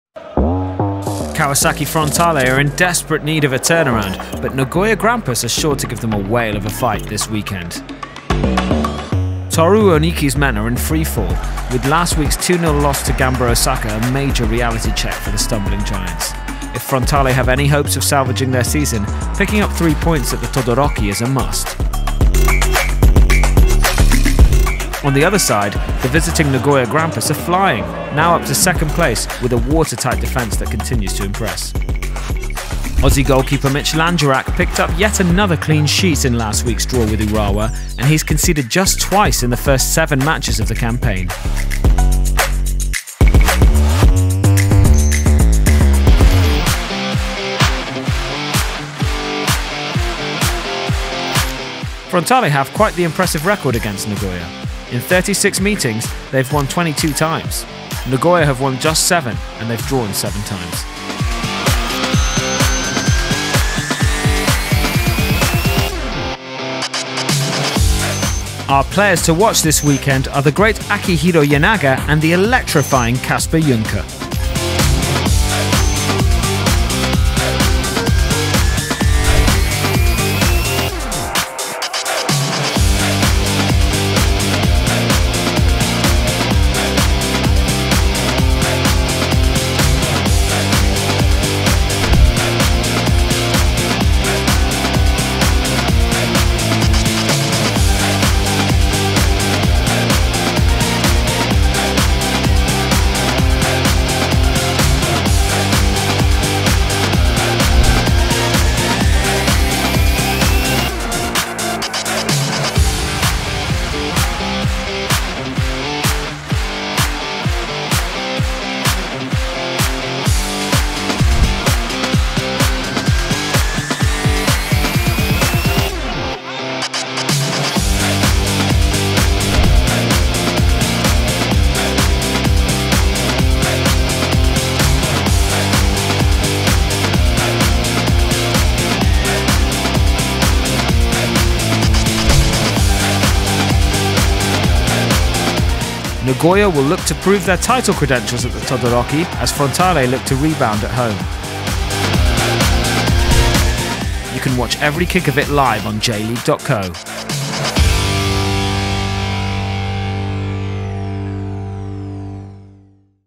Preview: Kawasaki Frontale vs Nagoya Grampus | Matchweek 8 | 2023 J1 League
Nagoya have their eyes on first place against stumbling Frontale.
Catch all the LIVE actions at 15:00 JST, Saturday, April 15th on J.LEAGUE International.